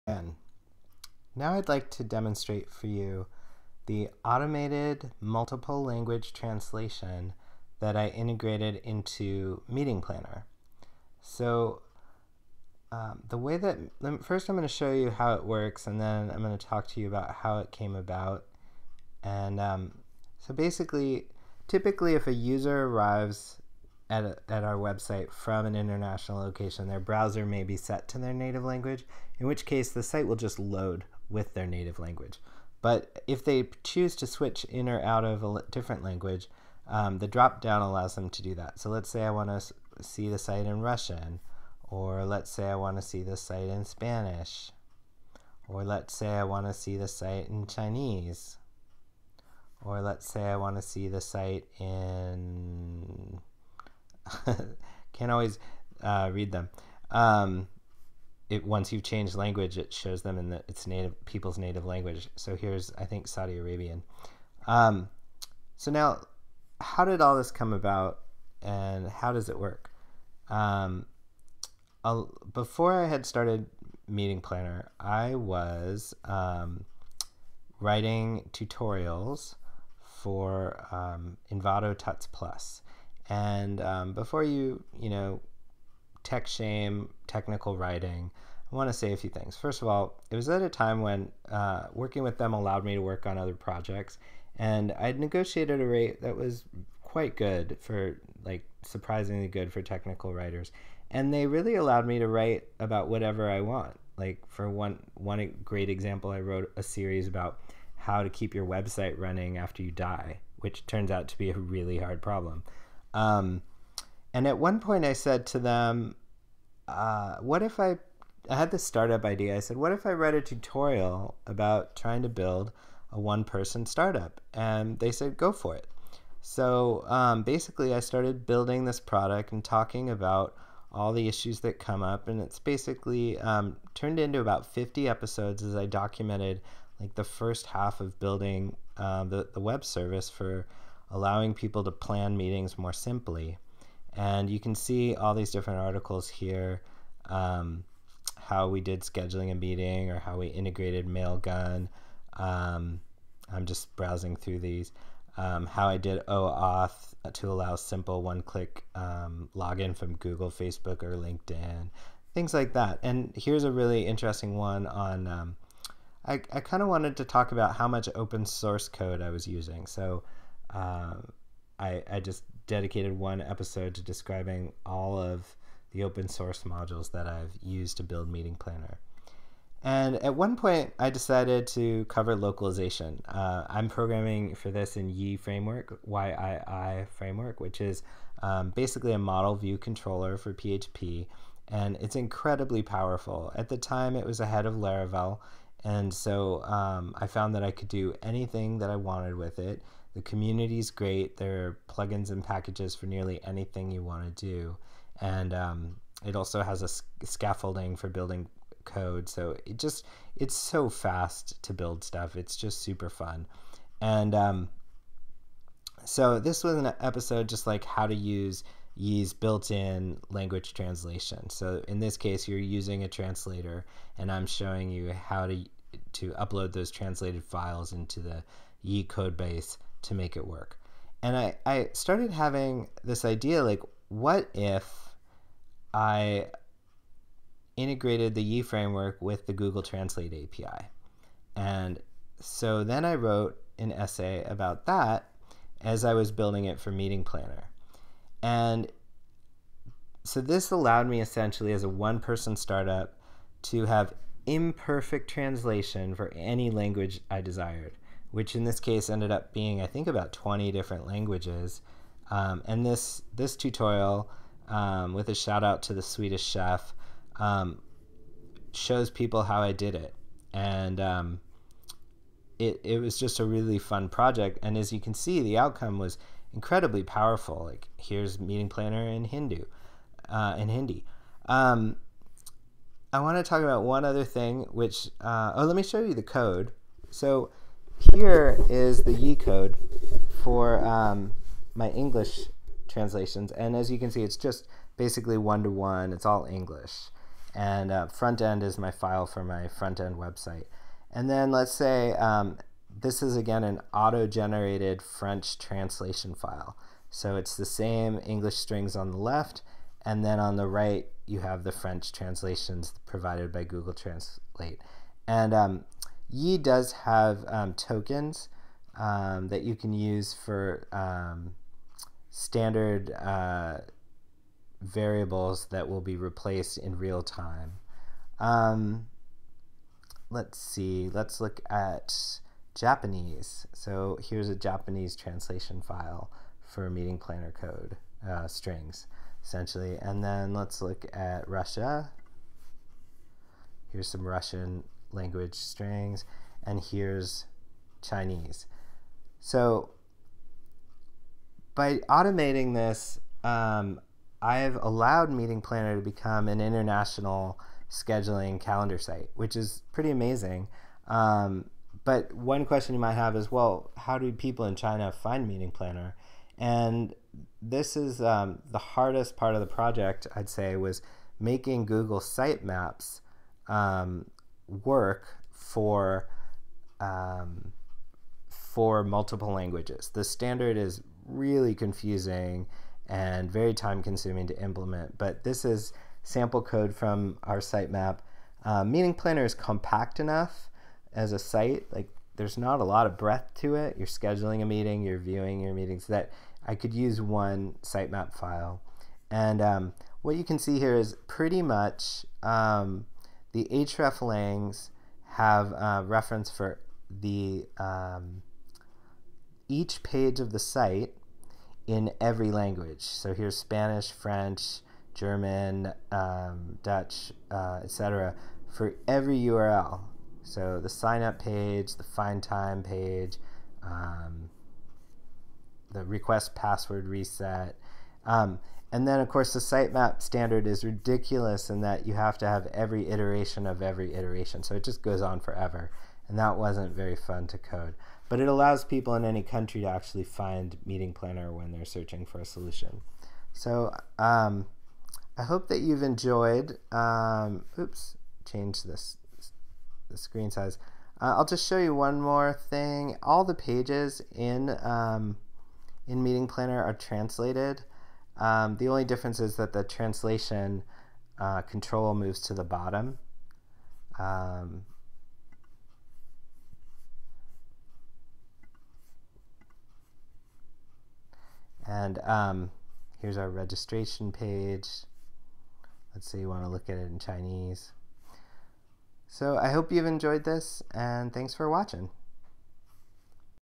Meeting Planner's Multiple Language Google Translate Integration with Yii Framework
While building Meeting Planner and writing programming tutorials for a startup series at Envato Tuts+, I wrote one about using the Yii Framework's translation files and I realized that imperfect translations could be automated by integrating them to Google Translate. This video demonstrates how Meeting Planner has imperfect translations for about 20 languages generated completely by Google Translate. The video shows language resource file examples as well as sitemaps that allow international users to discover the Meeting Planner service.